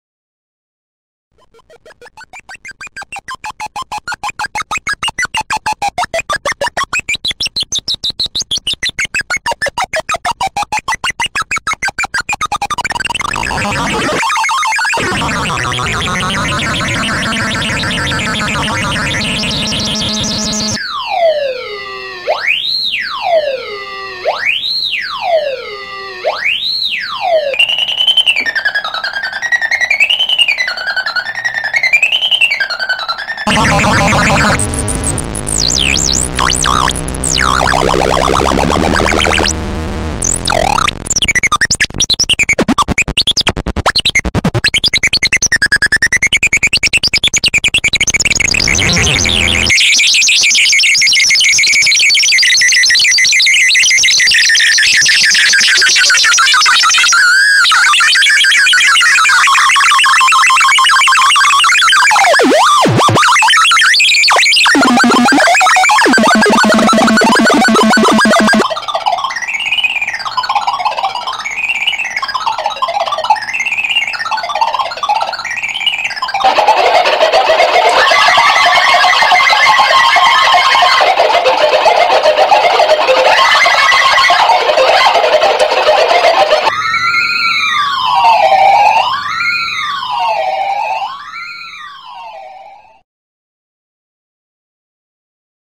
Bleep Labs Robo Audio Synths from ThinkGeek
Thingamagoop! Totally awesome sound machine for incredibly creative musical composition! Light sensitive and interactive!